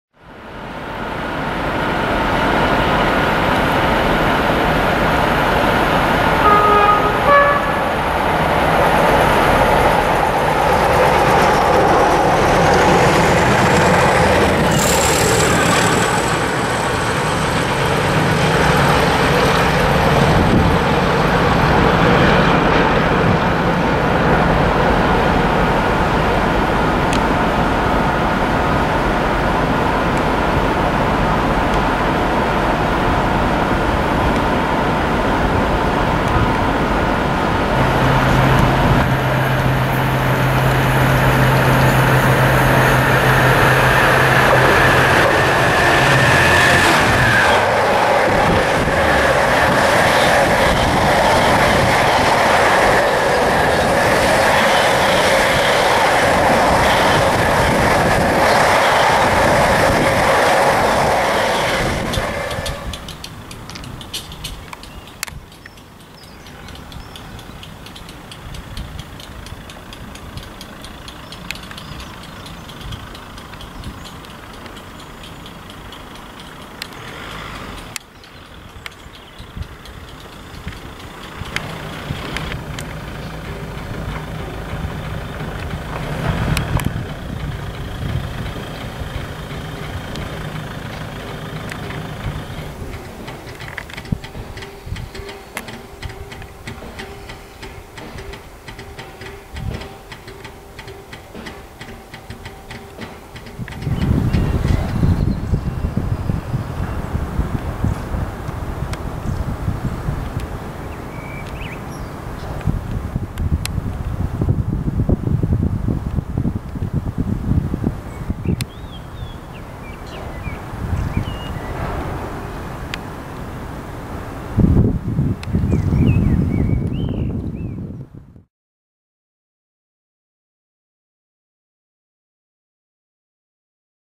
31601 + 50008 'Thunderer' 0Z31 at Warrington (plus Arpley Yard)
DCR ped 31601 drags Liara blue 50008 'Thunderer' on 0Z31 Washwood Heath Met. Cammel to Castleton Hopwood Gf on the East lancs railway. The locos are seen running 3L at 14:46 through Warrington with some toots and a bit of thrash.

Next up, DRS shed 66428 works the Darentry to Coatbridge FLT at 14:40.

A few shots of locos in Arpley yard consist of 67011, 66002, 60079 and MPV unit DR98962.